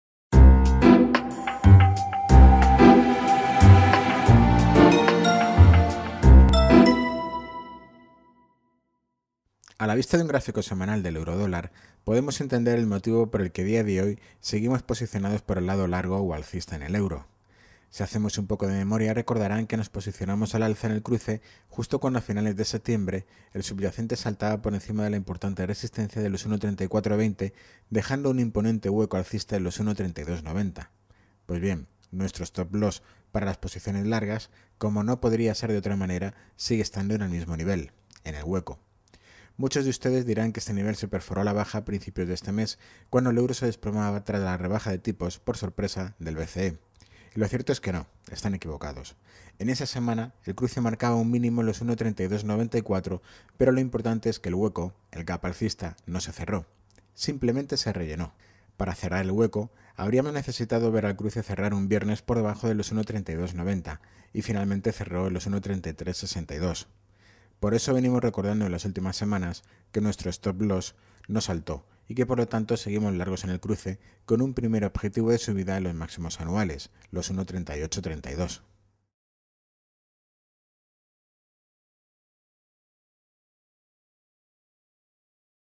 Euro/Dólar: ¿Por qué seguimos largos en el cruce?
A la vista del gráfico semanal es más fácil entender el motivo por el que decimos aquello de que, tendencialmente, seguimos largos en el euro. En la medida en que el hueco semanal de los 1,3290 siga vigente seguiremos largos en el cruce.